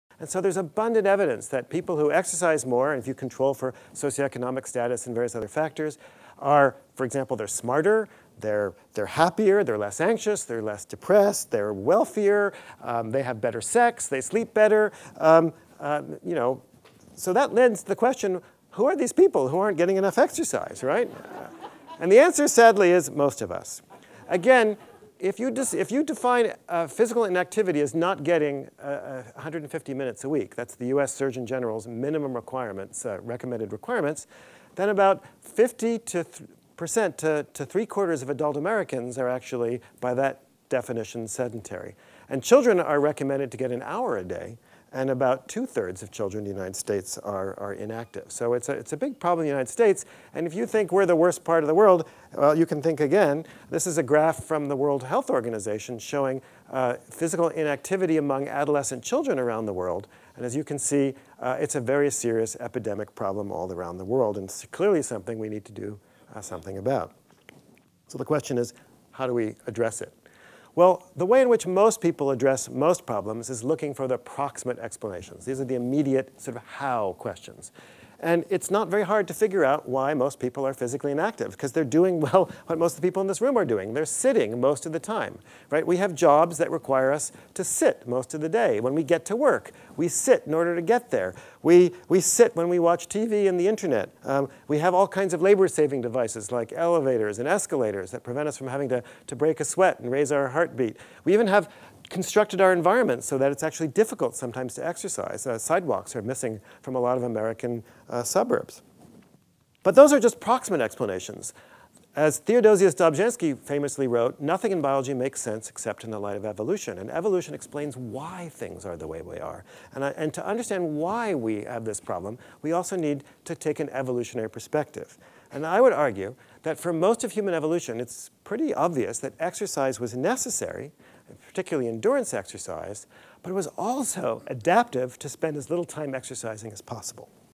Survival: Daniel Lieberman - Featured Clip
Everyone knows that exercise promotes health, but the lens of evolution is necessary to address why exercise is so vital for health and wellbeing, and why so many people today avoid exercise despite its benefits. An evolutionary perspective not only explains why inadequate exercise accelerates aging and contributes to many diseases, but also predicts that to help more people exercise in the post-industrial world it is important to make exercise both fun and necessary.

This clip is part of Daniel Lieberman's talk from the Survival symposium, a series of talks focusing on evolution and the challenges of building a better, safer and more survivable future.

About the speaker:
Daniel Lieberman is professor and chair of the Department of Human Evolutionary Biology and the Edwin M. Lerner II Professor of Biological Sciences at Harvard University. His research is on how and why the human body is the way it is, with particular foci on the origins of bipedalism, how humans became superlative endurance runners, and the evolution of the highly unusual human head. His latest books are "The Evolution of the Human Head (Harvard University Press, 2011), and “The Story of the Human Body” (Pantheon, 2013).